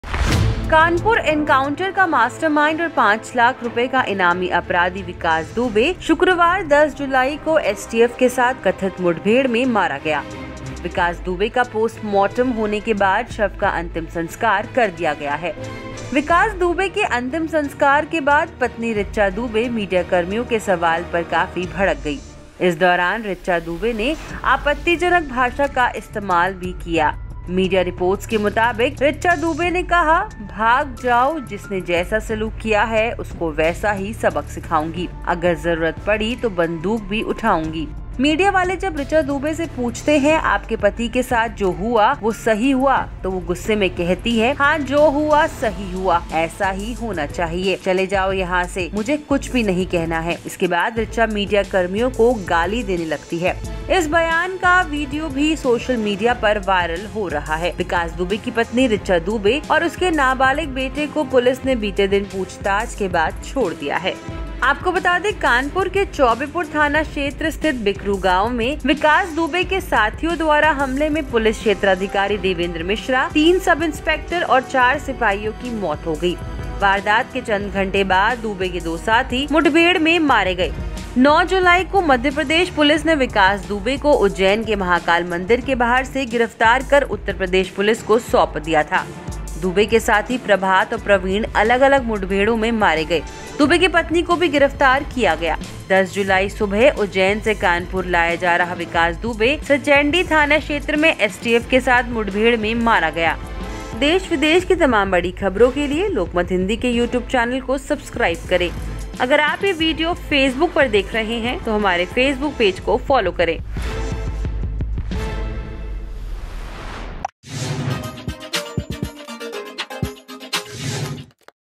Vikas Dubey Encounter: विकास दुबे का हुआ अंतिम संस्कार, पत्नी ने कहा- जरूरत पड़ी तो बंदूक भी उठाऊंगी
कानपुर के बिकरू कांड का मास्टरमाइंड और पांच लाख रुपये का इनामी अपराधी विकास दुबे शुक्रवार (10 जुलाई) को एसटीएफ के साथ कथित मुठभेड़ में मारा गया। विकास दुबे का पोस्टमॉर्टम होने के बाद शव का अंतिम संस्कार कर दिया गया। विकास दुबे के अंतिम संस्कार के बाद पत्नी ऋचा दुबे मीडियाकर्मियों के सवाल पर काफी भड़क गईं। इस दौरान ऋचा दुबे ने आपत्तिजनक भाषा का इस्तेमाल भी क‍िया। अमर उजाला में छपी रिपोर्ट के मुताबिक, ऋचा दुबे ने कहा,''भाग जाओ, जिसने जैसा सलूक किया है, उसको वैसा ही सबक सिखाऊंगी। अगर जरूरत पड़ी तो बंदूक भी उठाऊंगी।''
मीडिया वाले जब ऋचा दुबे से पूछते हैं, आपके पति के साथ क्या जो हुआ, वो सही हुआ...तो वह गुस्से में कहती है- 'हां जो हुआ सही हुआ, ऐसा ही होना चाहिए। चले जाओ यहां से। मुझे कुछ भी नहीं कहना है।' इसके बाद ऋचा मीडिया कर्मियों को गाली देने लगती हैं। इस बयान का वीडियो भी सोशल मीडिया पर वायरल हो रहा है। विकास दुबे की पत्नी ऋचा दुबे और उसके नाबालिग बेटे को पुलिस ने बीते दिन पूछताछ के बाद छोड़ दिया है।
विकास दुबे घटनाक्रम : जानें कानपुर शूटआउट से लेकर कथित मुठभेड़ में मारे जाने तक
3 जुलाई - कानपुर के चौबेपुर थाना क्षेत्र स्थित बिकरू गांव में विकास दुबे के साथियों द्वारा घात लगाकर किए गए हमले में पुलिस क्षेत्राधिकारी देवेंद्र मिश्रा, तीन सब इंस्पेक्टर और चार सिपाहियों की मौत। वारदात के चंद घंटे बाद दुबे के 2 साथी मुठभेड़ में मारे गए।
4 जुलाई- चौबेपुर के थाना अध्यक्ष विनय तिवारी को पुलिस की दबिश के बारे में जानकारी विकास दुबे को देने के संदेह में निलंबित किया गया। पुलिस ने दुबे के बिकरू गांव स्थित घर को जमींदोज किया।
हिस्ट्रीशीटर विकास दुबे और ऋचा दुबे (फाइल फोटो)
5 जुलाई -विकास दुबे का गुर्गा दयाशंकर अग्निहोत्री कानपुर में एक मुठभेड़ के बाद गिरफ्तार।
6 जुलाई -सरकार ने चौबेपुर थाने के तीन अन्य पुलिस कर्मियों को भी निलंबित किया। पुलिस ने कहा कि वह वारदात में मारे गए पुलिस उपाधीक्षक देवेंद्र मिश्रा के उस कथित पत्र में लगे आरोपों की जांच कर रही है जिनके तहत विकास दुबे और चौबेपुर के तत्कालीन थानाध्यक्ष विनय तिवारी के बीच संबंध होने की बात कही गई थी।
7 जुलाई -सरकार ने चौबेपुर थाने में तैनात सभी 68 पुलिसकर्मियों को लाइन हाजिर किया। दुबे के तीन और साथी गिरफ्तार हुए। कानपुर के निवर्तमान वरिष्ठ पुलिस अधीक्षक अनंत देव तिवारी एसटीएफ के उपमहानिरीक्षक पद से हटाकर पीएसी मुरादाबाद स्थानांतरित किए गए।
हिस्ट्रीशीटर विकास दुबे को गिरफ्तार कर ले जाती उज्जैन पुलित (फाइल फोटो)
8 जुलाई -विकास दुबे का एक और सहयोगी पुलिस के साथ मुठभेड़ में मारा गया और उसके छह अन्य साथी गिरफ्तार हुए।  चौबेपुर के निलंबित थानाध्यक्ष विनय तिवारी और निलंबित किए गए बीट इंचार्ज केके शर्मा गिरफ्तार हुए। एसटीएफ ने विकास दुबे के रिश्तेदार राजू निगम को मध्य प्रदेश के शहडोल जिले में गिरफ्तार किया।
9 जुलाई -मध्य प्रदेश पुलिस ने विकास दुबे को उज्जैन के महाकाल मंदिर के बाहर से गिरफ्तार कर उत्तर प्रदेश पुलिस को सौंपा। दुबे के साथी प्रभात और प्रवीण अलग-अलग मुठभेड़ों में मारे गए। दुबे की पत्नी को गिरफ्तार किया गया।
10 जुलाई -उज्जैन से कानपुर लाया जा रहा विकास दुबे सचेंडी थाना क्षेत्र में एसटीएफ के साथ मुठभेड़ में मारा गया।